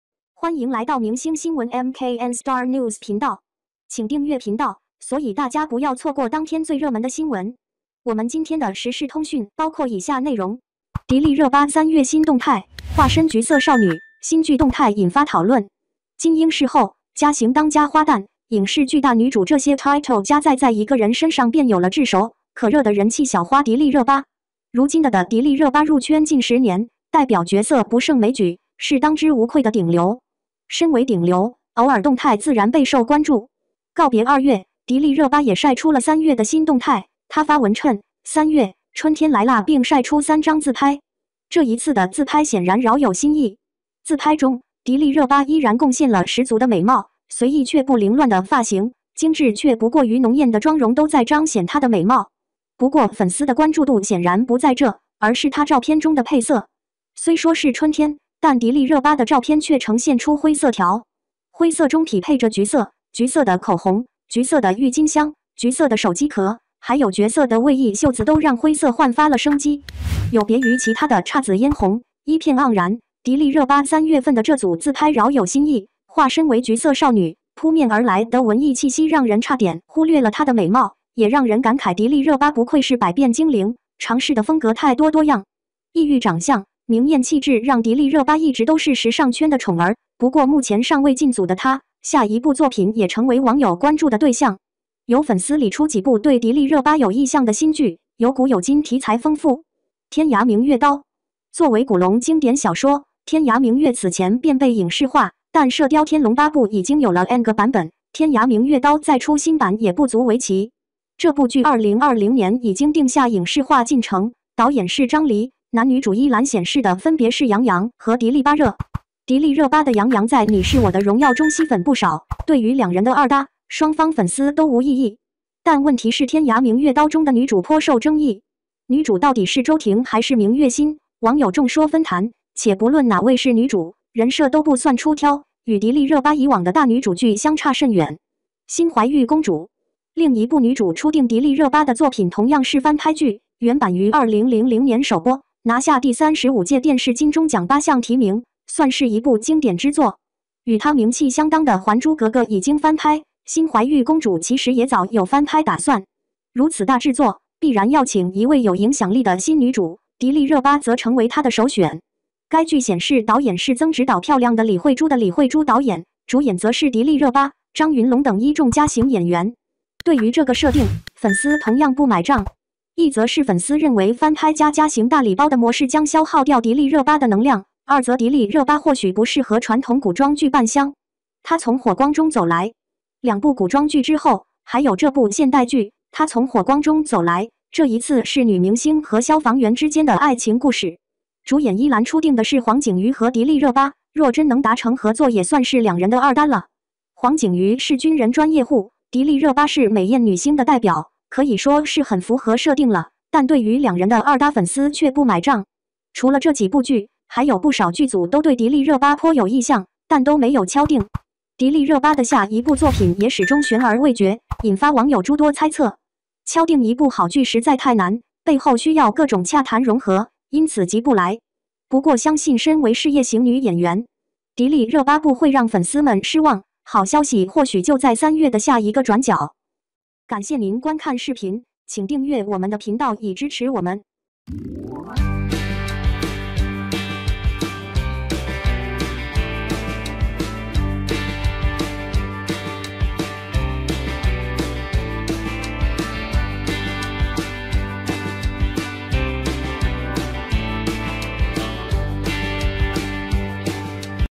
迪丽热巴3月新动态，化身“橘色少女”，新剧动态引发讨论
迪丽热巴3月新动态，化身“橘色少女”，新剧动态引发讨论  | MKN Star News.